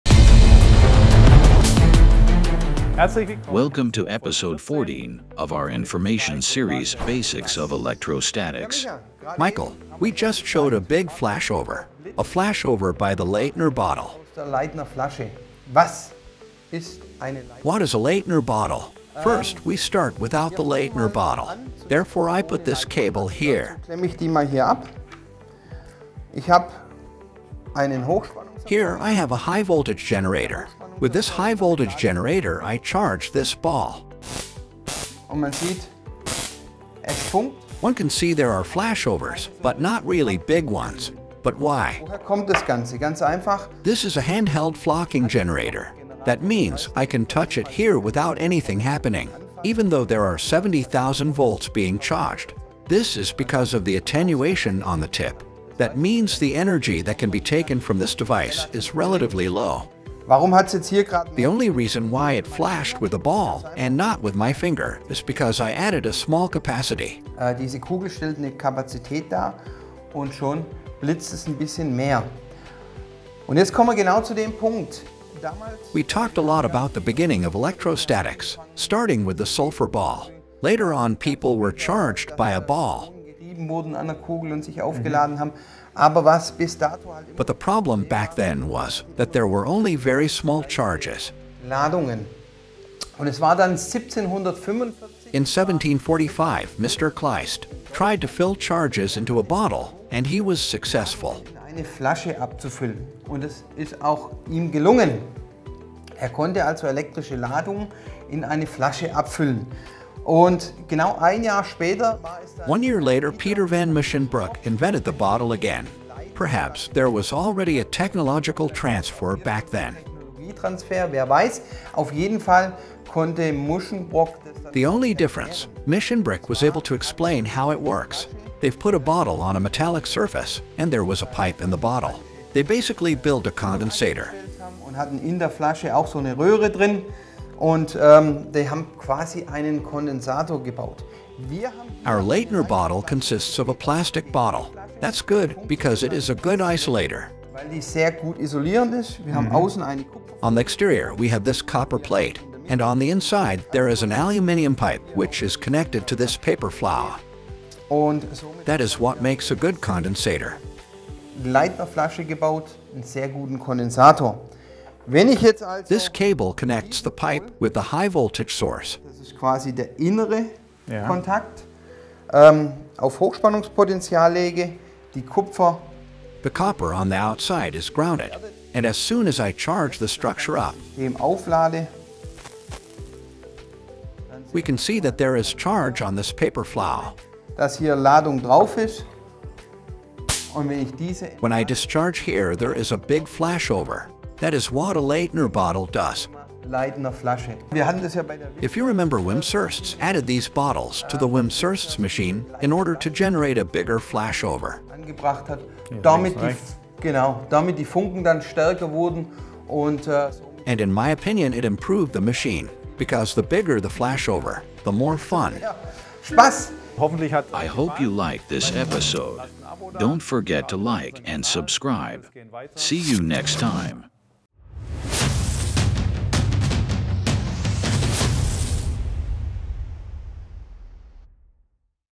Basics of Electrostatics - Episode 14: Leyden Jar (Leiden Jar, Kleistian Jar)
We show and explain the Leyden Jar (Leiden Jar, Kleistian Jar)

►►►►►👉 *_more information_* 👈◄◄◄◄◄

Welcome to episode 14 of our knowledge series "Basics of electrostatics".
With this series we want to share our know how about electrostatics with you to make electrostatic applications safer.